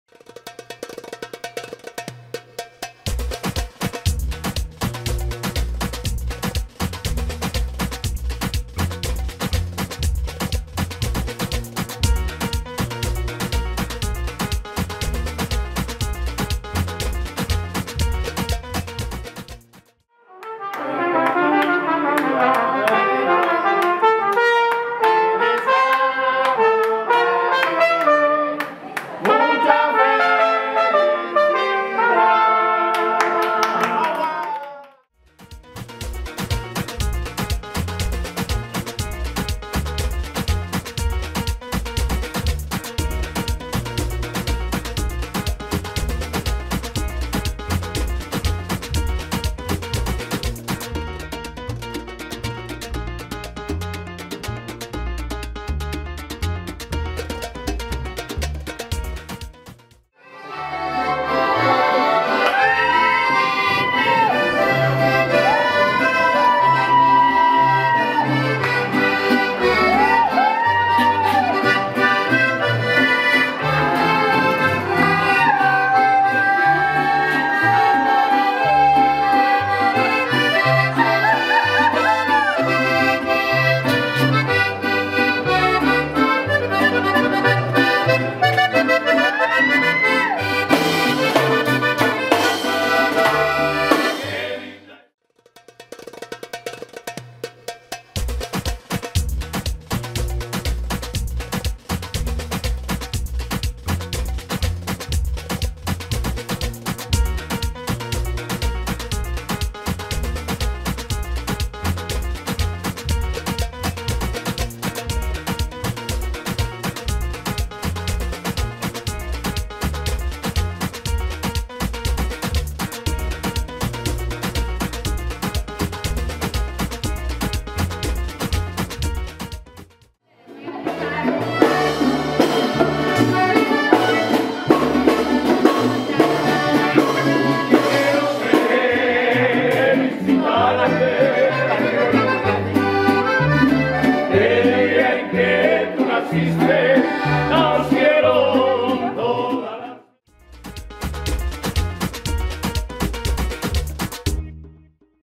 Raquel Bigorra celebró su cumpleaños rodeada de amor
El pasado 15 de junio AlejandroGavira, esposo de RaquelBigorra, la consintió por su cumpleaños 48. Durante el último ensayo de su show Havana Nights, un cachito de Cuba, llegaron varios seres queridos a la celebración.